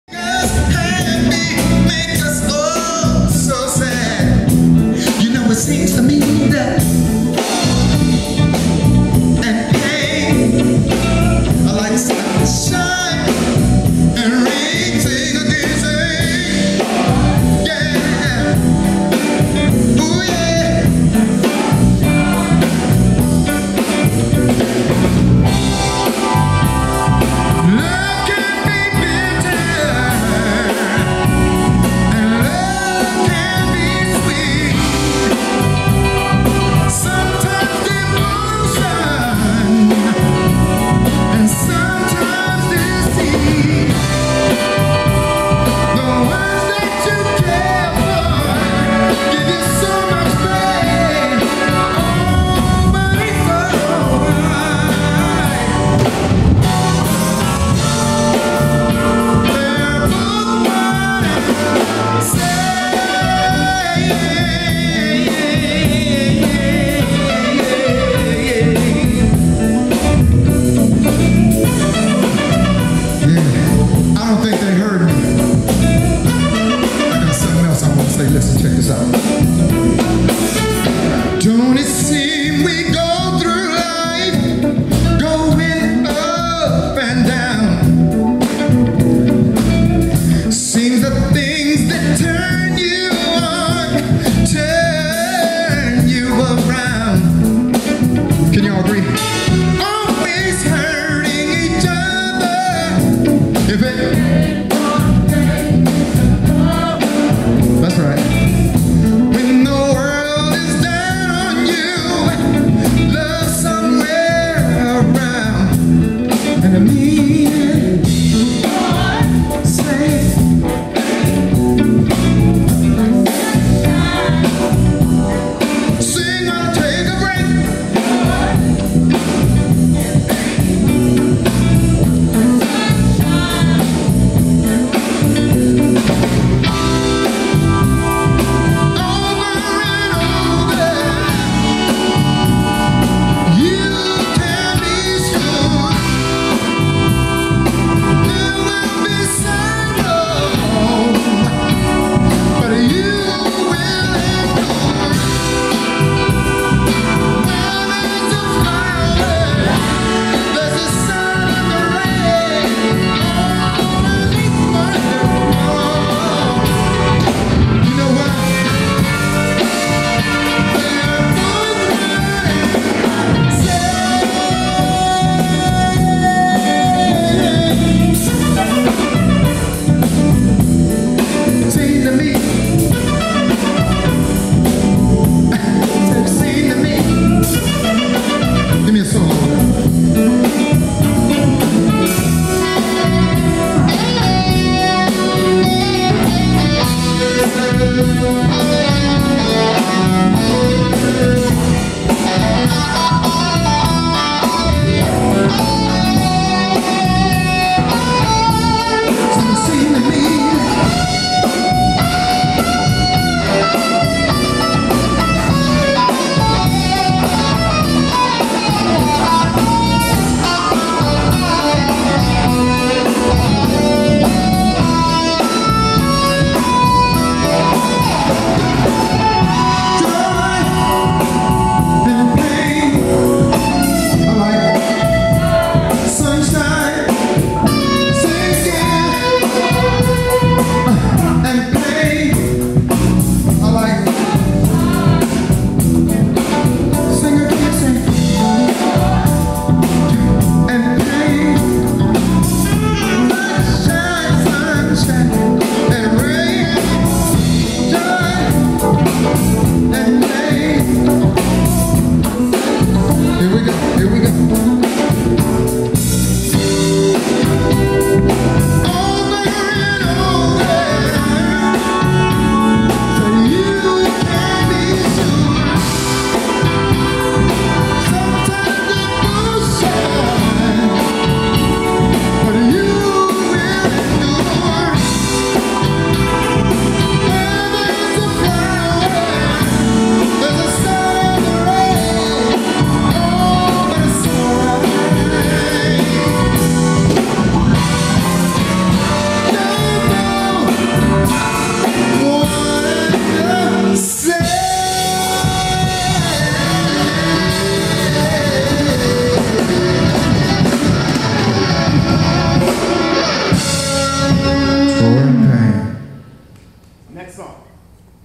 Rjai's Productions Presents The Positive Source Band
Hollywood Actor Robert Jamison presents The Positive Source Band Featuring Ricky Abernathy Former lead singer of Lakeside Together Forever Musical Tour...